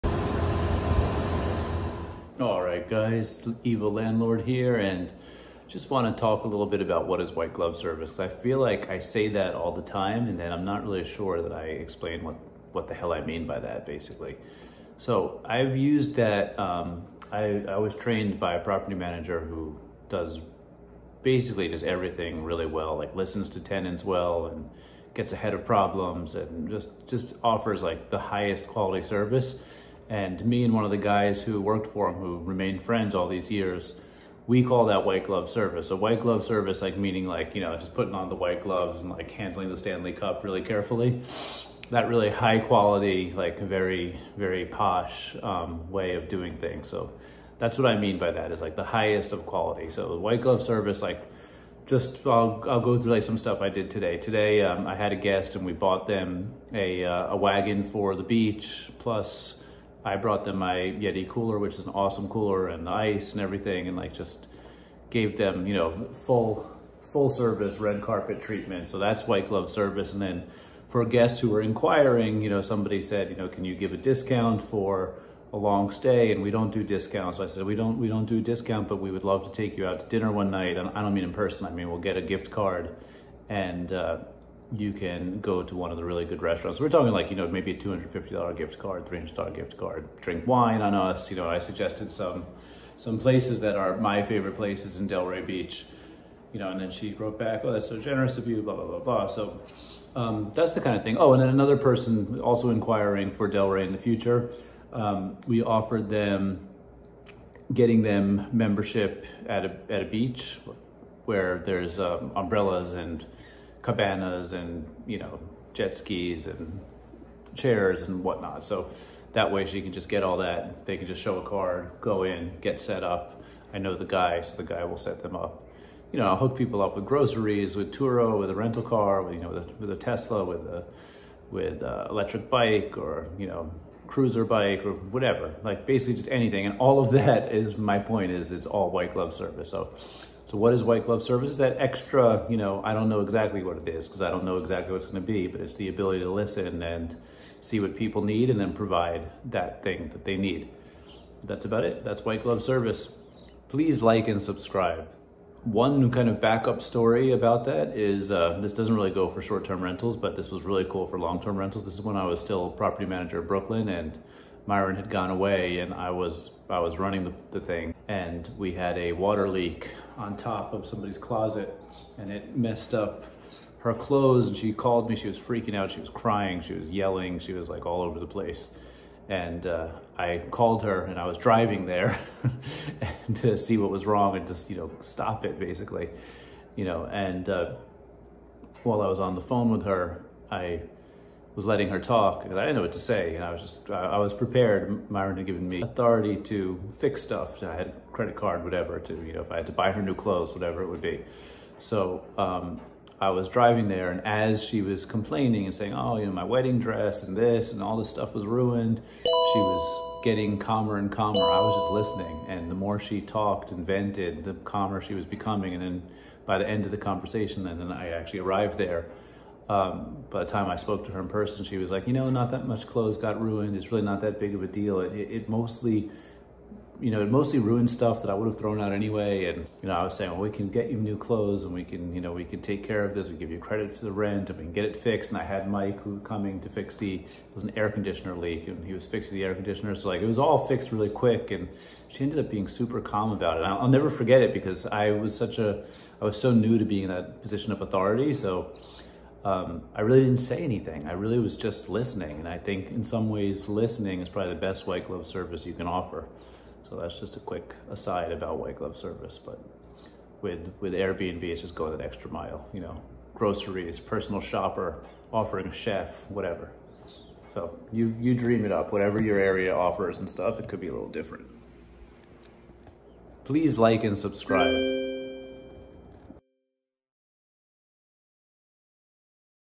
White Glove Service in Short term Rental Property Management — What is it?
White Glove Service in Short Term Rental Property Management | Ralph Chiaia

In this video, Ralph Chiaia, a Real Estate Agent and Investor on YouTube, delves into the concept of White Glove Service in short-term rental property management. Ralph defines White Glove Service as delivering the highest quality service to tenants, not just by fulfilling their requests but by anticipating their needs and going the extra mile.

Key Highlights:

 1. Definition of White Glove Service:
 • Providing top-tier service to tenants.
 • Anticipating and meeting tenant needs proactively.
 2. Example 1:
 • Ralph provided a guest with a wagon for the beach, a cooler with ice, and restaurant recommendations.
 • Instead of offering a discount for a long stay, he gifted the guest a restaurant gift card.
 3. Example 2:
 • Ralph offered a future guest in Delray Beach a membership at a beach club with amenities like umbrellas, cabanas, jet skis, and chairs.
 • He also connected guests with groceries, rental cars, Teslas, electric bikes, or cruiser bikes.
 4. The Importance of Listening:
 • Ralph shares a story from his time as a property manager in Brooklyn, where he de-escalated a situation by calmly listening to a tenant vent about a water leak that ruined her clothes.

Overall, this video emphasizes that White Glove Service is about providing exceptional service by anticipating tenant needs, going the extra mile, and actively listening to their requests.

Watch the video to learn how to elevate your property management skills and provide unparalleled service to your tenants!